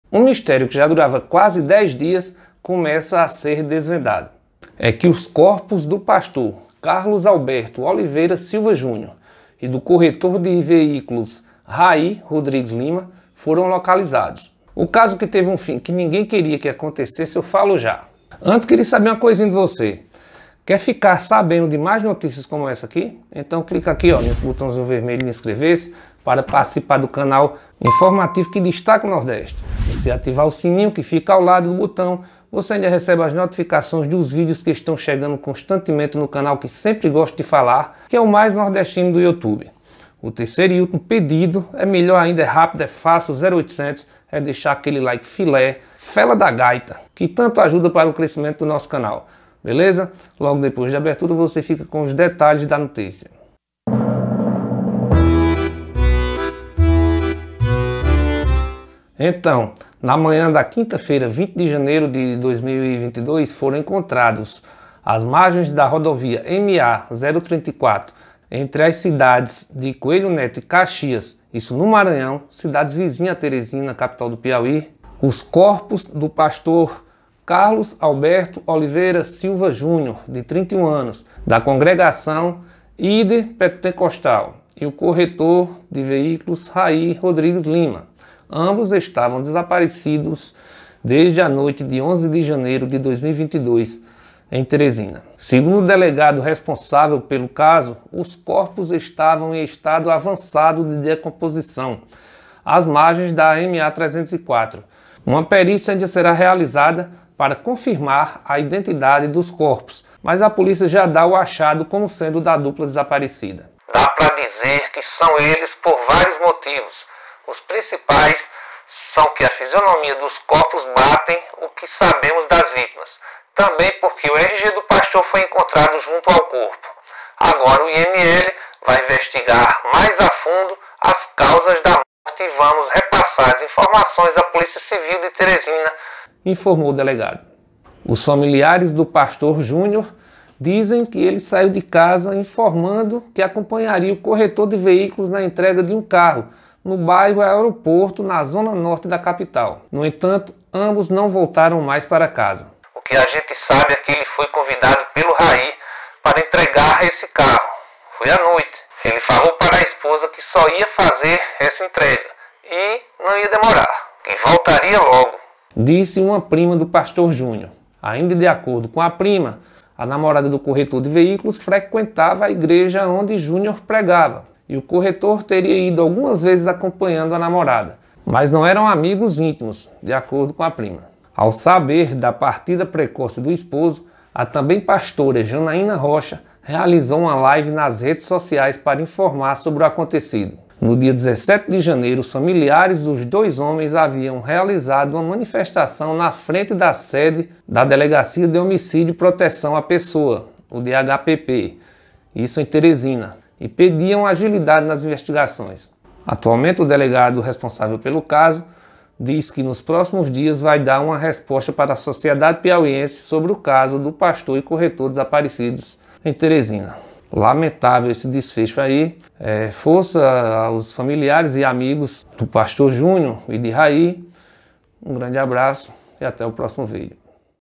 Corpos de pastor e corretor desaparecidos em Teresina PI são achados | Coelho Neto MA | Caxias MA
Na manhã da quinta-feira, 20 de janeiro, foram encontrados às margens da rodovia MA-034, entre as cidades de Coelho Neto e Caxias, no Maranhão, cidades vizinhas a Teresina, capital do Piauí, os corpos do pastor Carlos Alberto Oliveira Silva Júnior, de 31 anos, da Congregação IDE Pentecostal, e o corretor de veículos Raí Rodrigues Lima. Ambos estavam desaparecidos desde a noite de 11 de janeiro de 2022, em Teresina.

Segundo o delegado responsável pelo caso, os corpos estavam em estado avançado de decomposição, às margens da MA-304. Uma perícia ainda será realizada para confirmar a identidade dos corpos, mas a polícia já dá o achado como sendo da dupla desaparecida.

"Dá pra dizer que são eles por vários motivos. Os principais são que a fisionomia dos corpos batem o que sabemos das vítimas, também porque o RG do pastor foi encontrado junto ao corpo. Agora o IML vai investigar mais afundo as causas da morte e vamos repassar as informações à Polícia Civil de Teresina", informou o delegado.

Os familiares do pastor Júnior, dizem que ele saiu de casa informando que acompanharia o corretor de veículos na entrega de um carro, no bairro Aeroporto, na Zona Norte da capital. No entanto, ambos não voltaram mais para casa.

“O que a gente sabe é que ele foi convidado pelo Raí para entregar esse carro. Foi à noite. Ele falou para a esposa que só ia fazer essa entrega, e não ia demorar, que voltaria logo”, disse a prima do pastor Júnior.

Ainda de acordo com a prima do pastor, a namorada do corretor de veículos frequentava a igreja onde Júnior prega, e o corretor teria ido algumas vezes acompanhando a namorada. “Mas eles não eram amigos, íntimos. Isso podemos garantir”, afirmou Jéssica.

No dia 17 de janeiro, os familiares dos dois homens haviam realizado uma manifestação na frente da sede do Delegacia de Homicídio e Proteção à Pessoa (DHPP), em Teresina, pedindo respostas e pediam agilidade nas investigações.

O delegado contou ainda que nos próximos dias vai dar um resposta para a sociedade piauiense sobre o caso do pastor e corretor desaparecidos em Teresina.